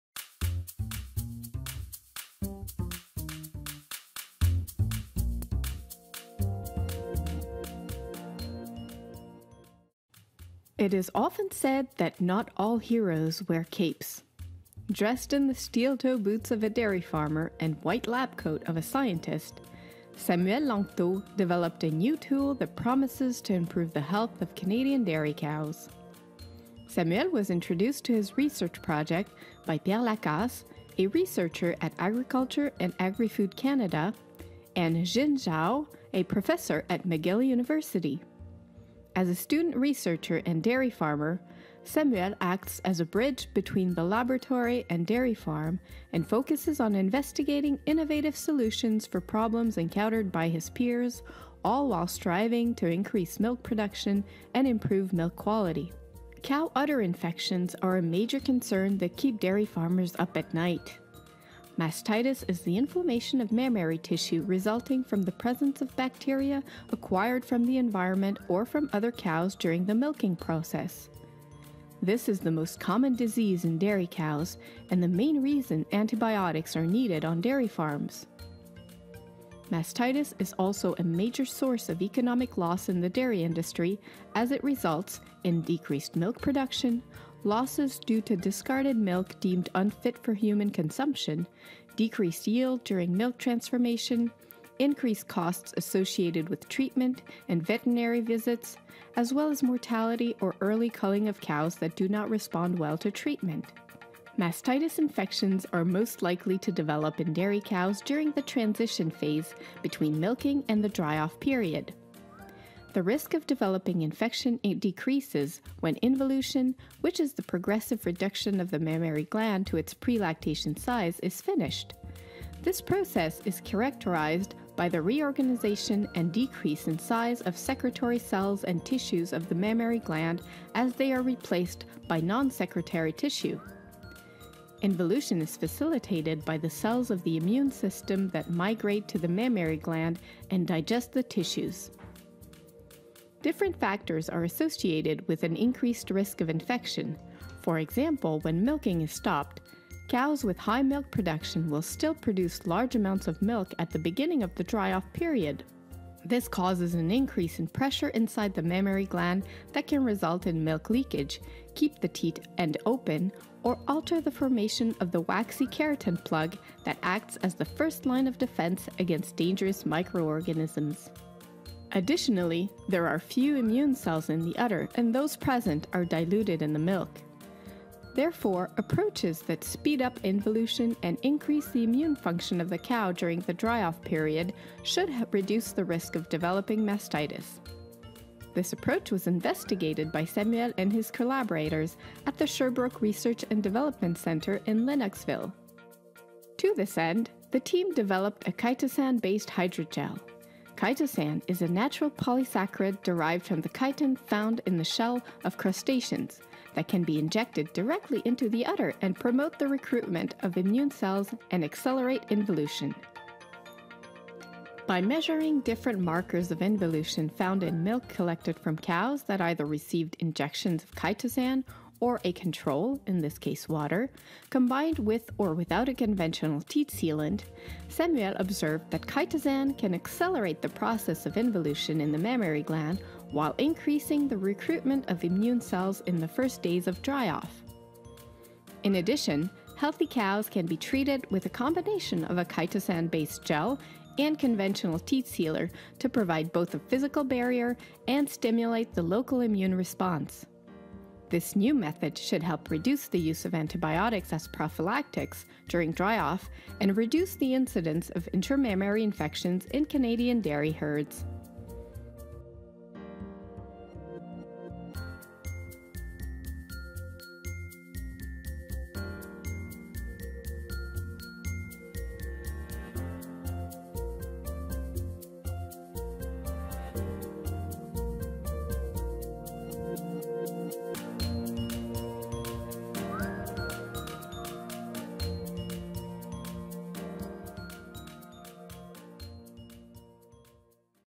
From shellfish to udder health
Samuel Lanctôt describes how chitosan, an extract from the shells of shellfish, can help prevent intramammary infections in dairy cows.